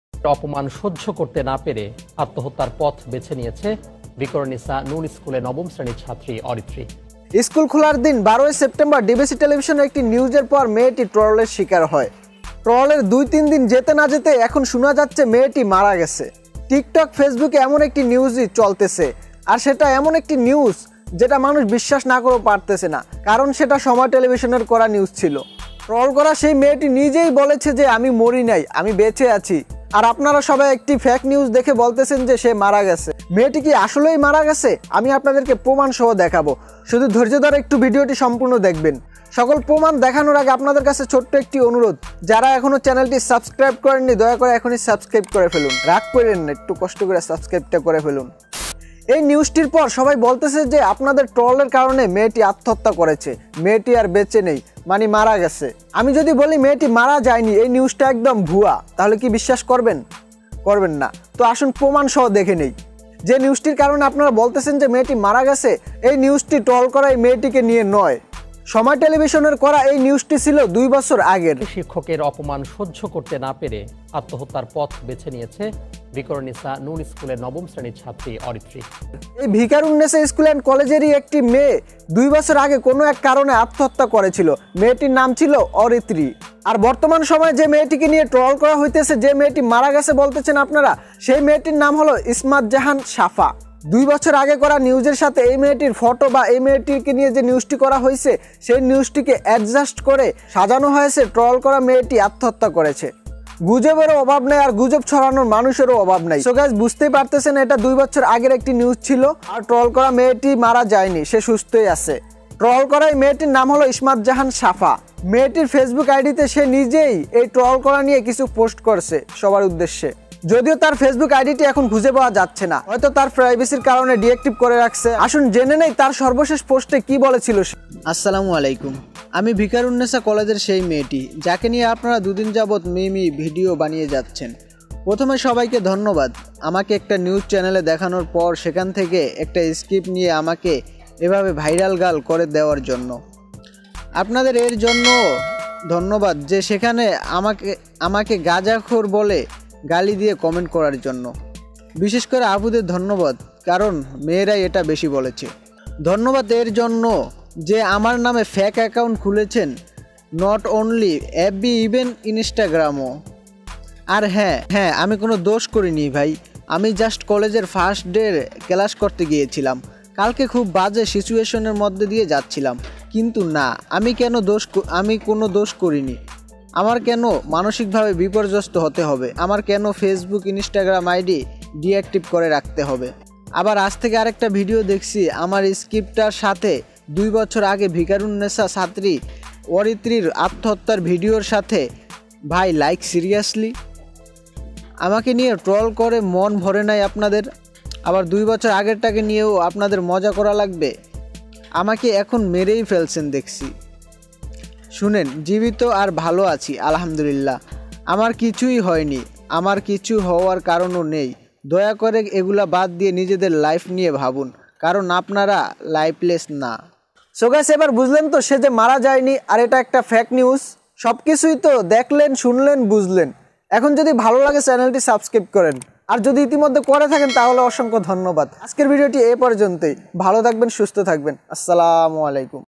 মেয়েটি আর নেই প্রমানসহ দেখুন | টিকটক ফেসবুকে ট্রল করা কে এই মেয়ে | ভিকারুননিসা | ismat jahan safa
Ismat jahan safa troll girl on Facebook and tiktok
Viqarunnisa noon school and College Dhaka.